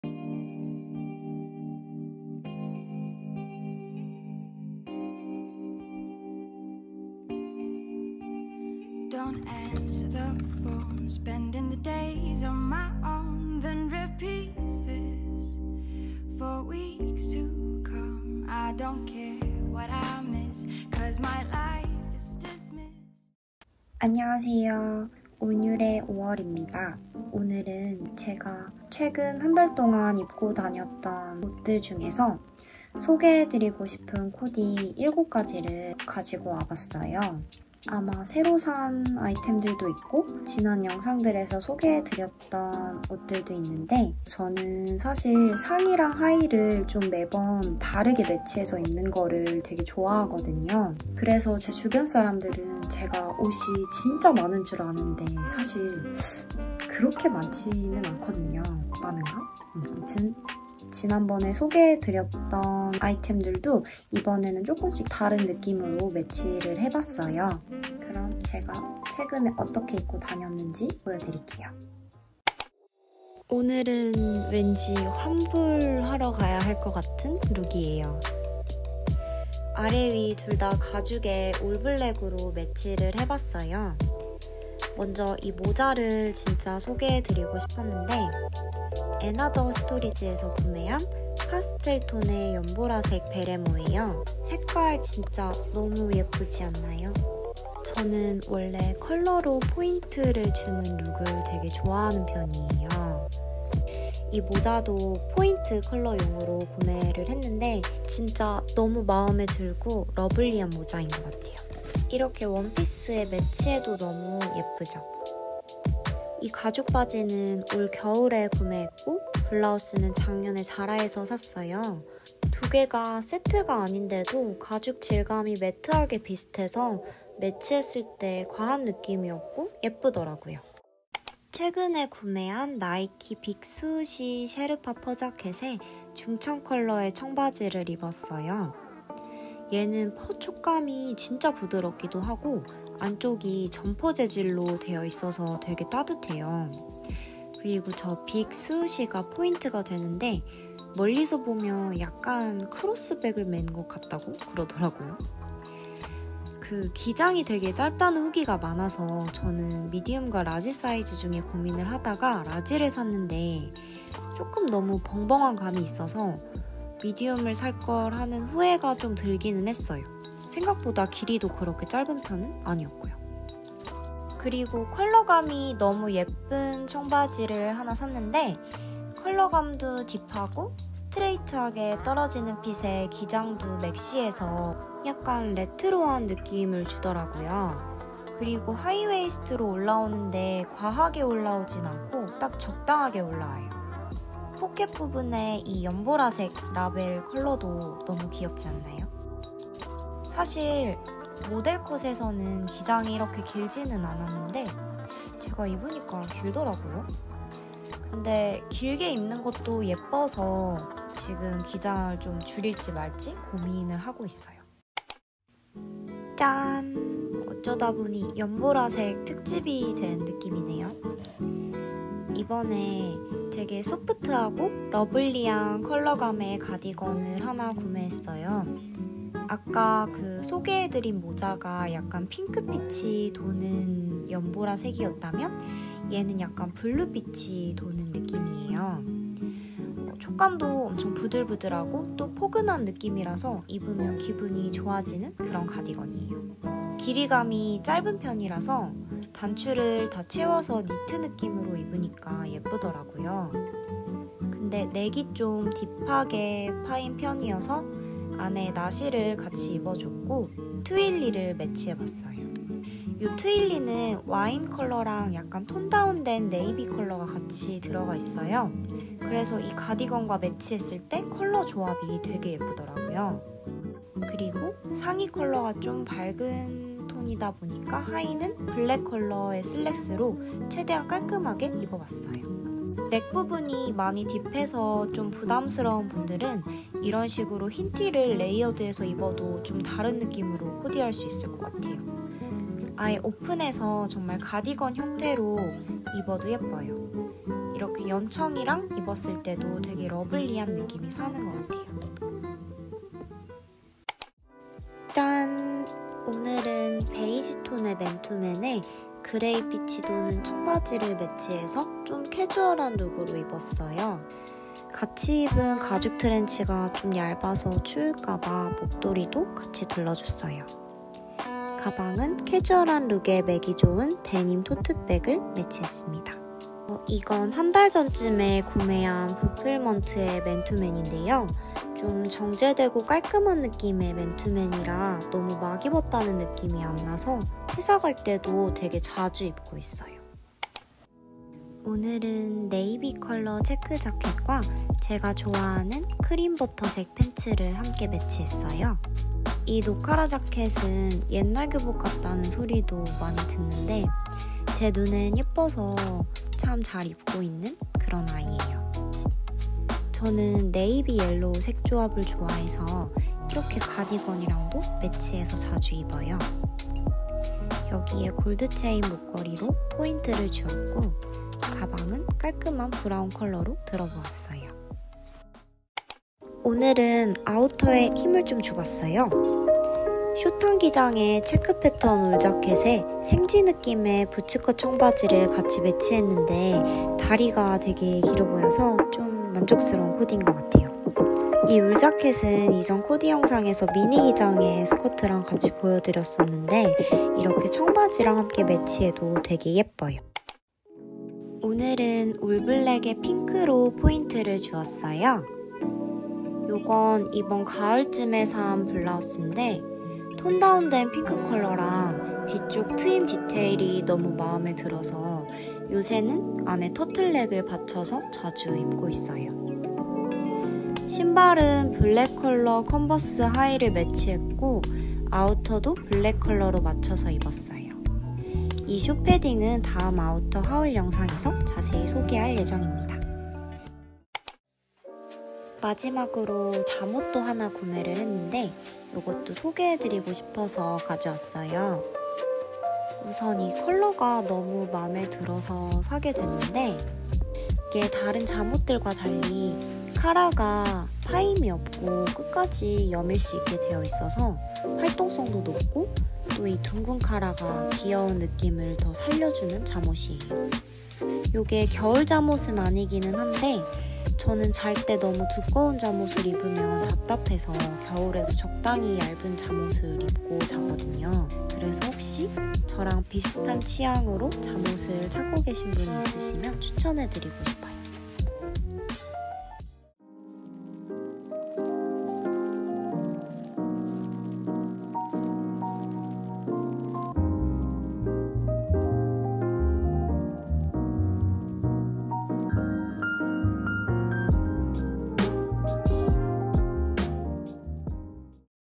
(Eng) ⛄🕺🏻 𝙒𝙞𝙣𝙩𝙚𝙧 𝙡𝙤𝙤𝙠𝙗𝙤𝙤𝙠 | 164cm 데일리룩 ootd | 나이키 퍼자켓, 앤아더스토리즈, ZARA, A.P.C, 이자벨마랑
안녕하세요! 오뉼입니다 🥰
오늘은 오월이의 첫 단독 데일리룩 영상을 준비했어요!
새로산 아이템들과 지난 영상에서 소개해드렸던 옷들로
같은 옷 다른 코디를 보여드리려고 합니다👀

영상에 등장한 아이템들 정보는 아래에 적어두었어요 ❣
품절이나 온라인 구매링크가 없는 제품들은 따로 적지 않았습니다..!

Day 1
blouse | ZARA
hat | 앤아더스토리즈 / 35,000

Day 2
outer | NIKE / 빅스우시 셰르파 퍼자켓 / 199,000
jeans | OBLA / HIGH WAIST MENIM 100 -MID BLUE / S / 59,000

Day 3
cardigan | ZARA / 케이블 니트 재킷 / 49,000

twilly scarf |  hermes

Day 4
sweat shirts | vpplement / 위켄드 스웻 맨투맨 - light beige / 69,000

muffler | Acne studios
bag | A.P.C

Day 5
jacket | 프롬비기닝 (*현재 품절인 상품이에요 :-/ )
pants | 프롬비기닝 / 로에브 핀턱 와이드 슬랙스 - cream butter  / 42,000

bag | Ecrit / HALF MOON BAG - SUEDE BROWN / 98,000

Day 6
jacket | mgmg studio (*지난 시즌 제품이에요 :-/)
pants | 리브어리 / 리모아 생지데님 / 45,000

Day 7
outer | ZARA / 패딩 점퍼 / 109,000

blouse | 어반로지 / 루카즈 절개 블라우스 - pink / 38,000
bag | Isabel Marant 이자벨마랑

Pajamas
하이홀릭 / 둥근카라 체크 파자마 / 34,000

👚오월 size
164 / 48 / S / 55